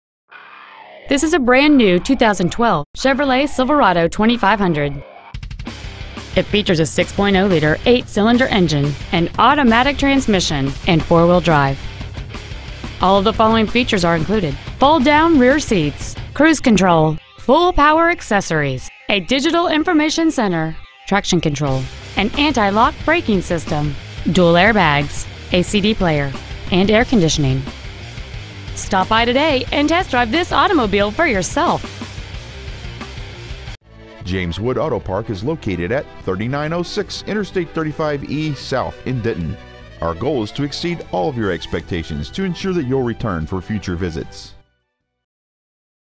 2012 Chevrolet Silverado 2500 Ft. Worth TX
Year : 2012
 Make : Chevrolet
 Model : Silverado 2500
 Engine : 6.0L 8 Cyl. Sequential Fuel Injection
 Trans . : 6 Speed Automatic
 Exterior : Summit White
 Miles : 10

 Stock : 223160

 2012 Chevrolet Silverado 2500 Denton TX